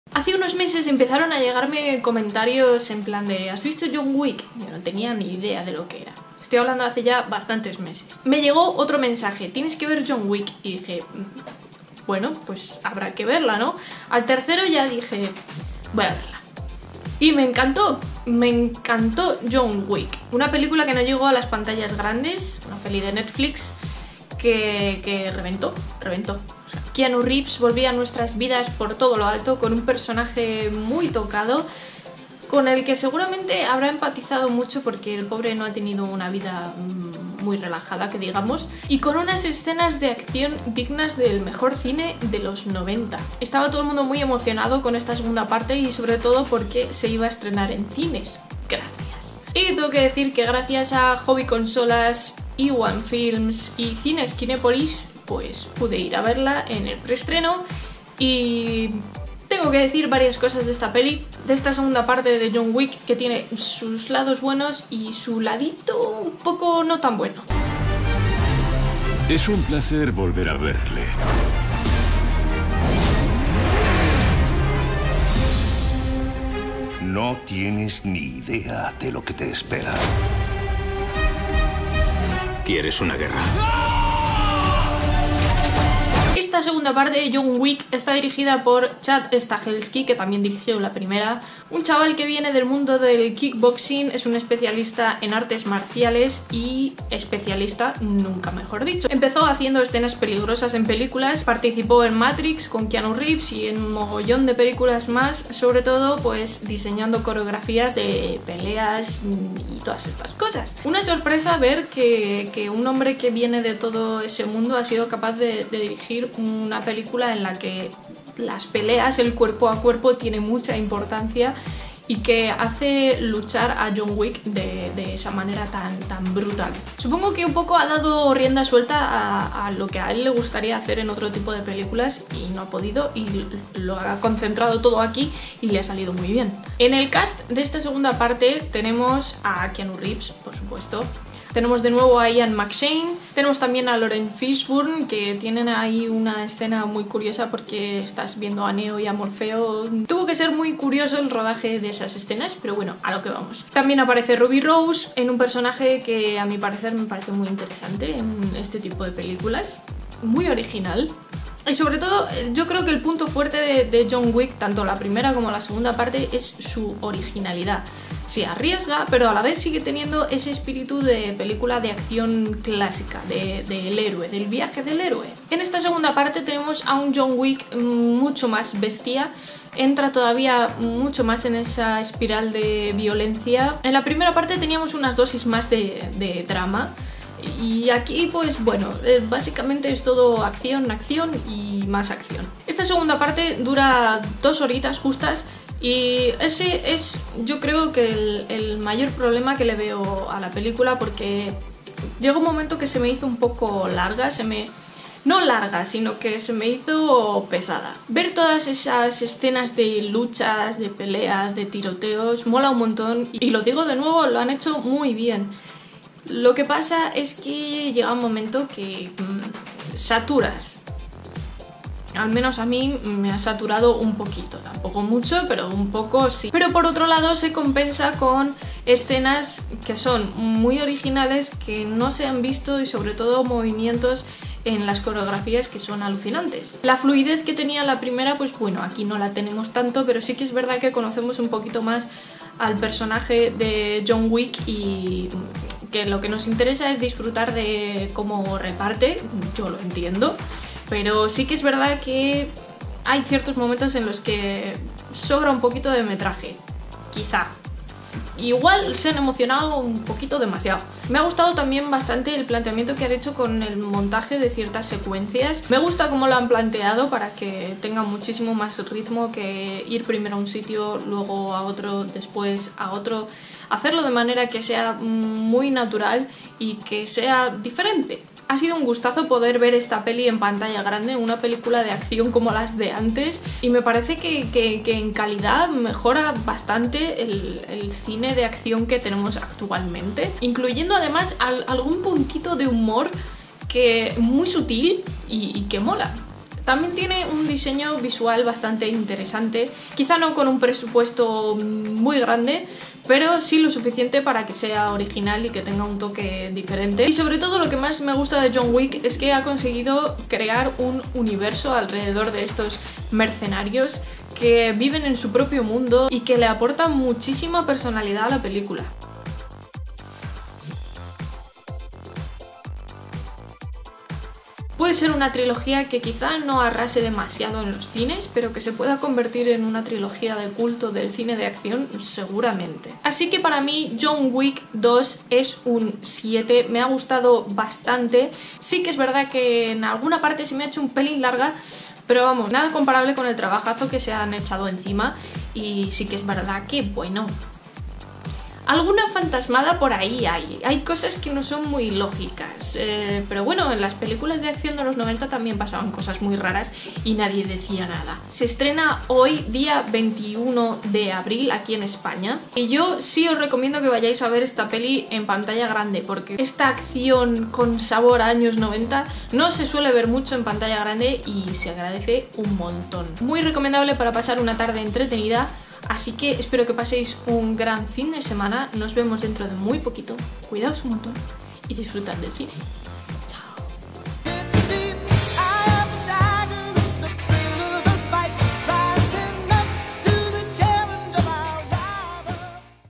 Crítica - 'John Wick 2. Pacto de sangre'
Keanu vuelve para repartir, y mucho XD